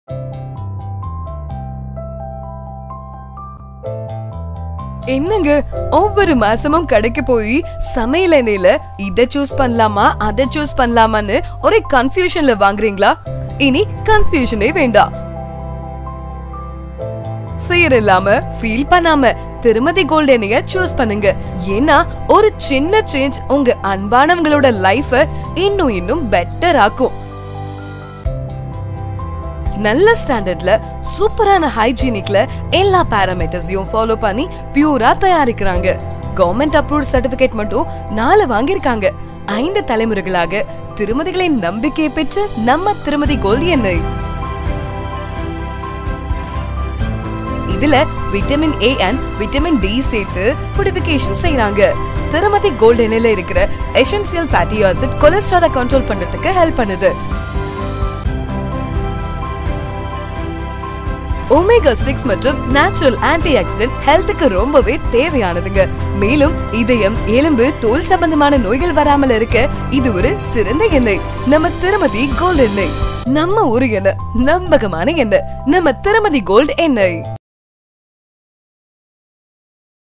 THIRUMATHI GOLD OIL || ADVERTISEMENT || TRUSTED OIL || RAJHAM REFINERIES
நம்ம ஊரு எண்ணெய்! நம்பகமான எண்ணெய்!
அது நம்ம திருமதி கோல்டு எண்ணெய்.
 அன்வுகுடியவர்களின் நலனுக்கு வாங்கவும்
நம்ம திருமதி கோல்டு எண்ணெய்!...